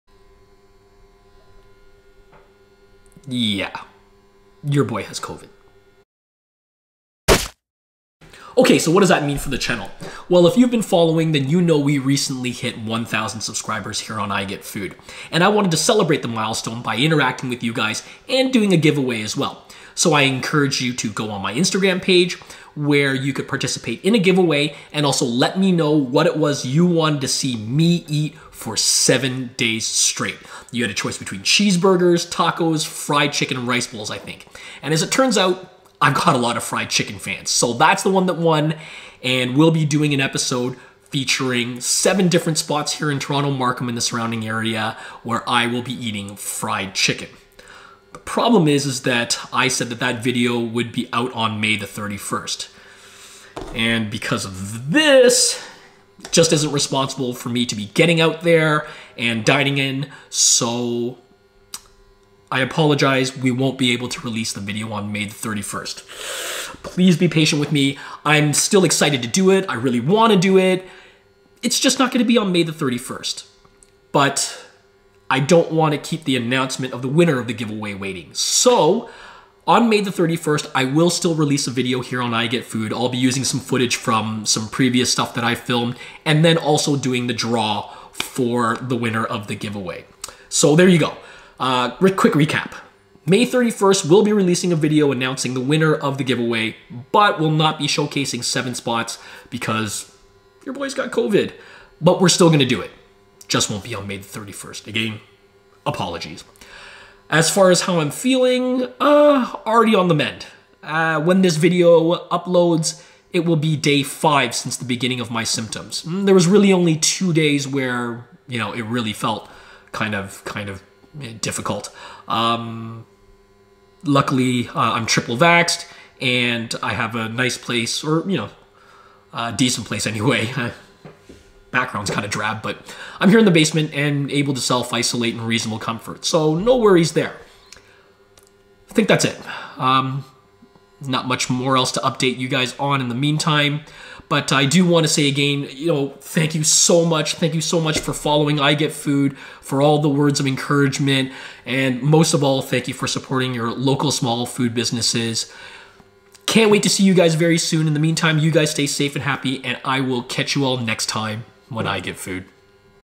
So Yeah, I Got Covid...And What That Means For My Upload Schedule
Just a quick announcement on how it will affect the upload schedule and the  giveaway.

I''m already on the mend and will resume showcasing local spots when it become responsible to do so. Peace!